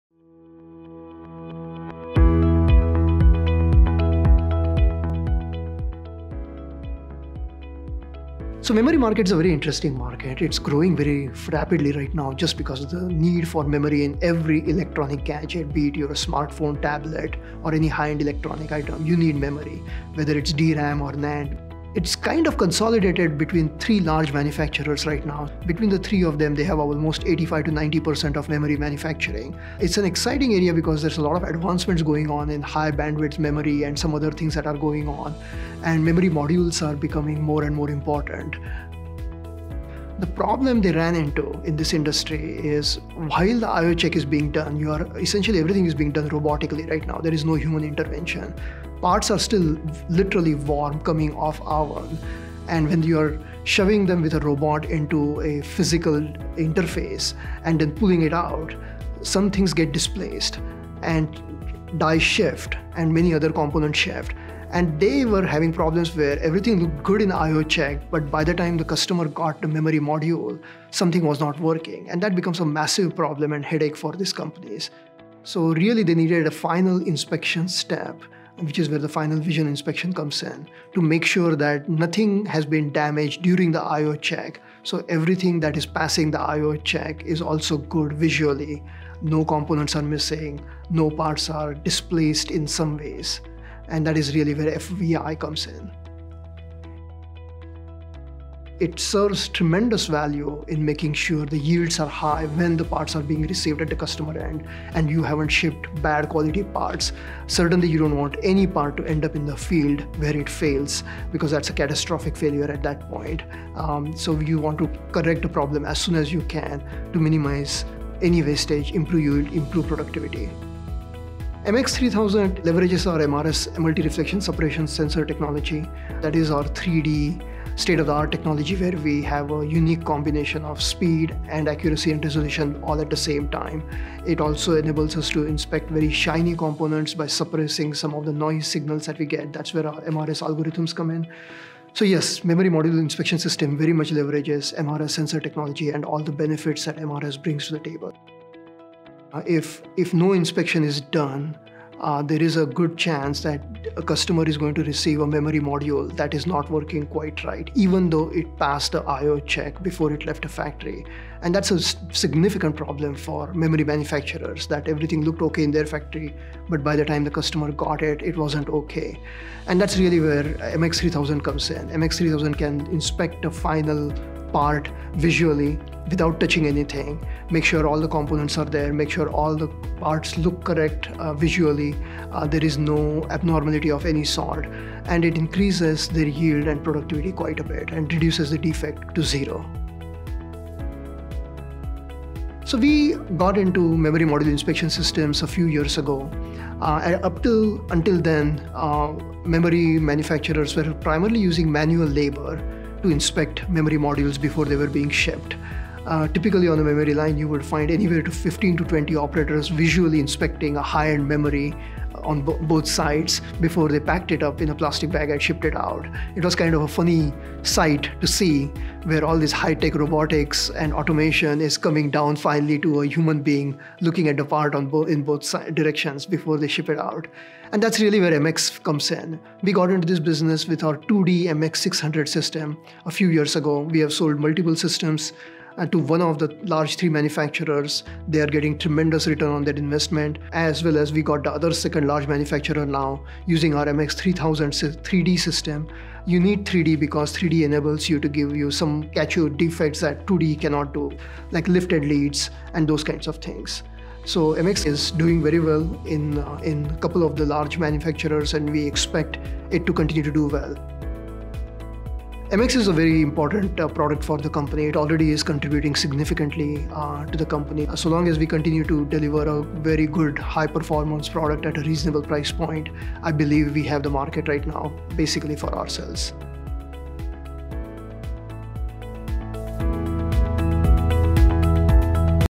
Q&A; Final Vision Inspection for Memory Modules | CyberOptics
Dr. Subodh Kulkarni, President and CEO, CyberOptics, sits down for a Q&A to discuss the market dynamics of the memory module market and how the MX3000™ 3D Memory Final Vision Inspection system provides a unique combination of high resolution, high accuracy and high speed to improve yields and productivity.